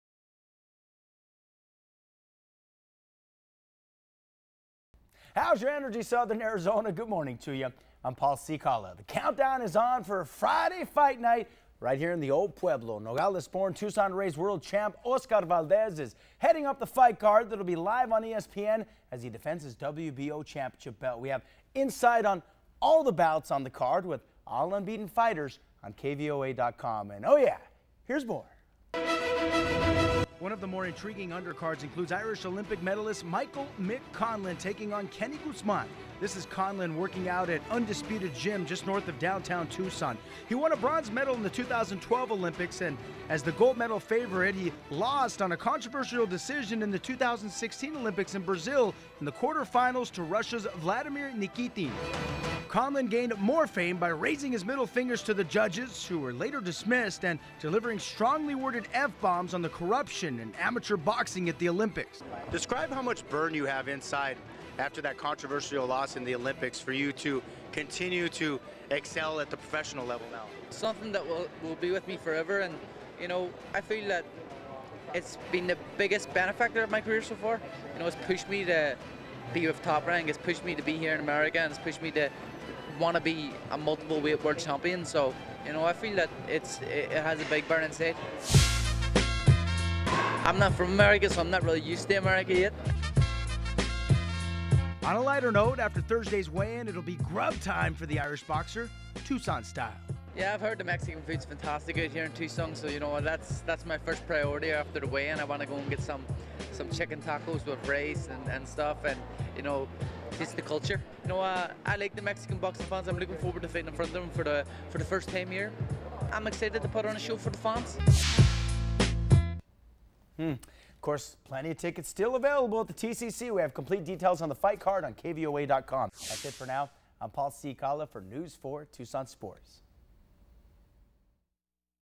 Irish Olympian's inner fire after being robbed by judges in Rio
News 4 Tucson's Paul Cicala goes 1-on-1 with Michael "Mick" Conlan in Southern Arizona as he gears up for his bout on Friday, Sept. 22nd at the TCC (on the ESPN Fight card).                  TUCSON, AZ (September 15, 2017) -- Undefeated World Boxing Organization (WBO) world champions ÓSCAR VALDEZ and GILBERTO "Zurdo" RAMIREZ will headline and exciting world championship event  Next Friday! September 22, at Tucson Arena (260 South Church Ave., Tucson, AZ 85701.)  Valdez (22-0, 19 KOs), a two-time Mexican Olympian with strong roots in Tucson, will be defending his WBO featherweight title against the WBO's No. 4 world-rated contender GENESIS SERVANIA (29-0, 12 KOs), of Bacolod City, Philippines.  WBO super middleweight champion Ramirez (35-0, 24 KOs), from Mazatlan Mexico, will take on No. 1 contender and mandatory challenger JESSE "Hard Work" HART (22-0, 18 KOs), of Philadelphia, PA, in the grudge match of the year.  The four warriors boast a perfect combined record of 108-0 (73 KOs) with nearly 70% of their victories coming by way of knockout.

The event will be further enhanced with the addition of Irish Olympic hero MICHAEL "MICK" CONLAN, 2016 U.S. Olympian MIKAELA MAYER and the battle for the vacant NABF welterweight title between EGIS KAVALIAUSKAS and MAHONRI MONTES.  Conlan (3-0, 3 KOs) will rumble in six-round featherweight bout against Kenny Guzman (3-0, 1 KO), Kalispell, Montana, and Mayer (1-0, 1 KO) will take on Allison Martinez (1-2, 1 KO) from Spring, Texas, in a four-round lightweight bout.